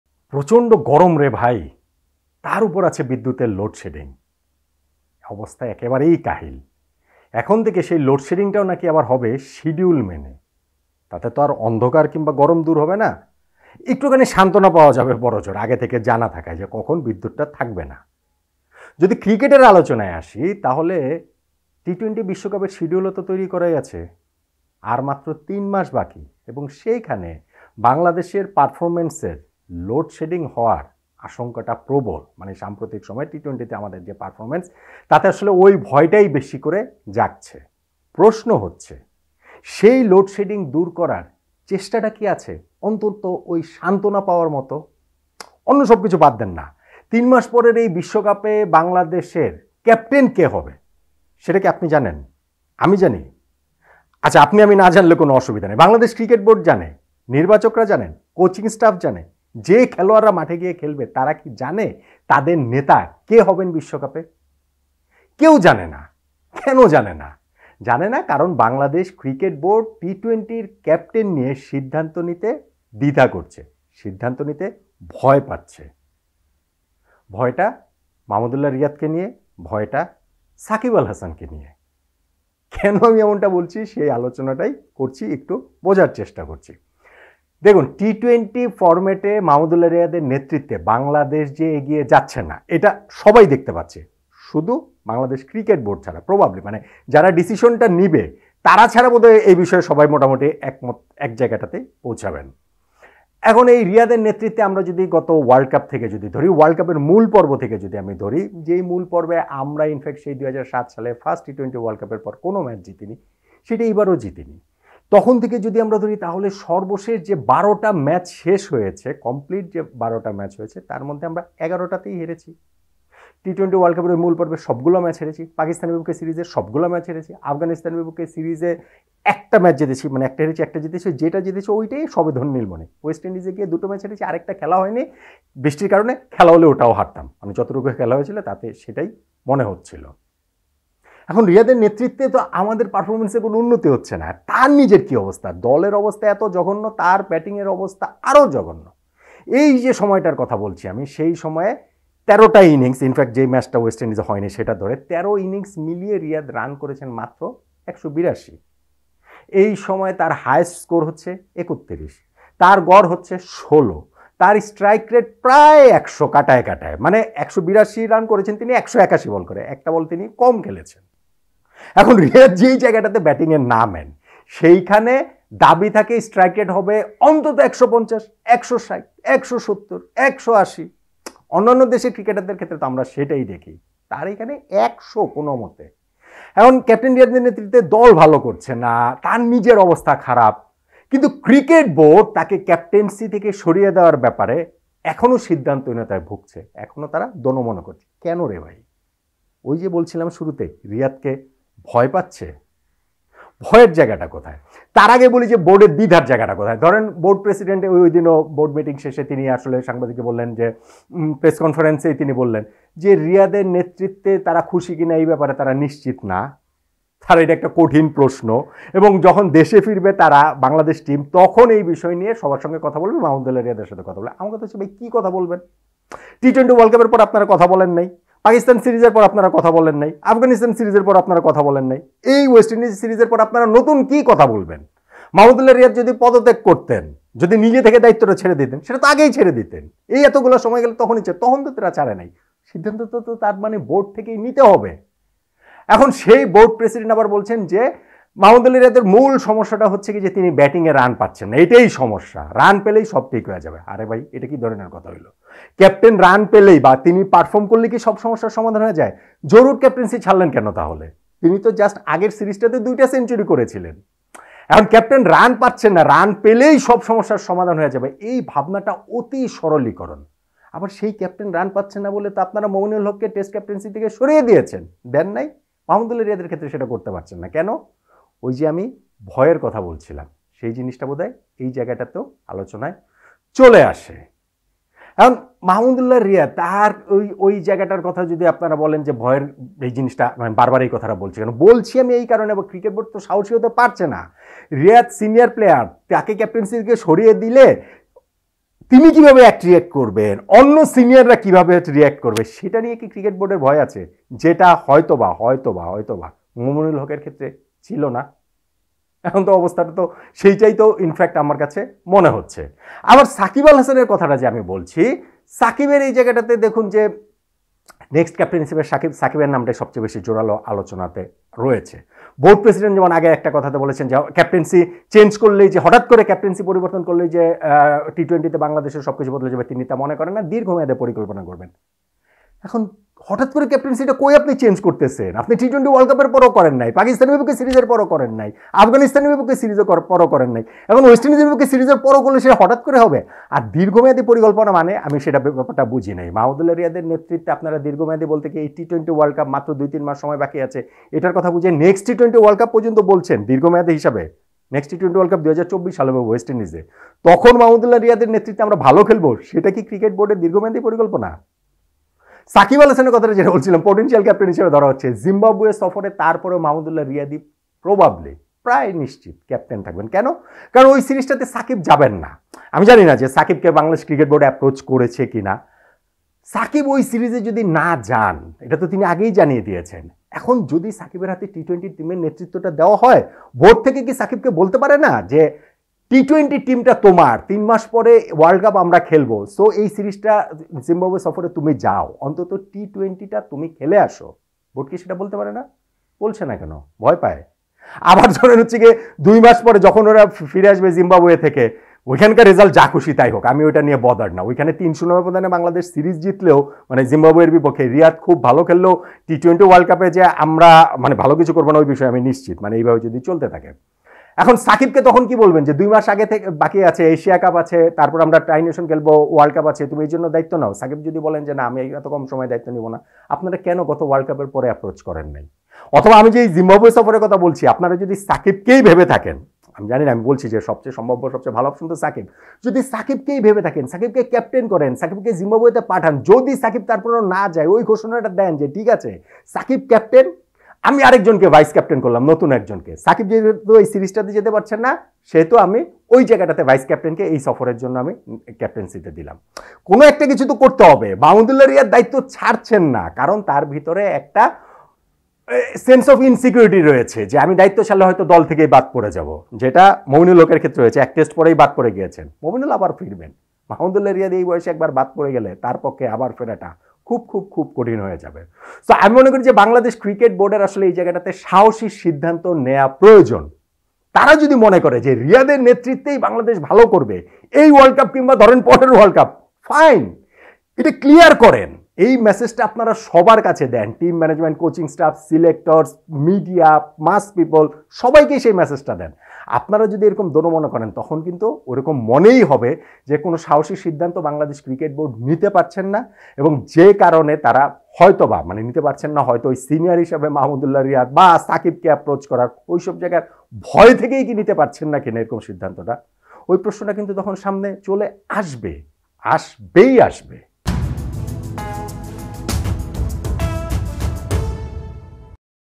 ক্যাপ্টেন পরিবর্তনে ভয়টা কোথায় বিসিবির? - নট আউট নোমান
টি-টোয়েন্টি বিশ্বকাপের বাকি মাত্র ৩ মাস। সেখানে বাংলাদেশের অধিনায়ক থাকবেন কে? কেউ জানে না। বাংলাদেশ ক্রিকেট বোর্ড যে এ নিয়ে দ্বিধায়, সেটি বিসিবি প্রেসিডেন্ট নাজমুল হাসান পাপনের কথাতে স্পষ্ট। তা সাম্প্রতিক সময়ে মাহমুদ উল্লাহ রিয়াদের নেতৃত্বে বাংলাদেশের রেজাল্ট ভয়াবহ, তাঁর নিজের ব্যাটিংও যাচ্ছেতাই -- তাহলে ক্যাপ্টেন পরিবর্তনে ভয়টা কোথায় বিসিবির? রিয়াদকে ভয়? সাকিবকে ভয়? নট আউট নোমান-এ সে আলোচনাই।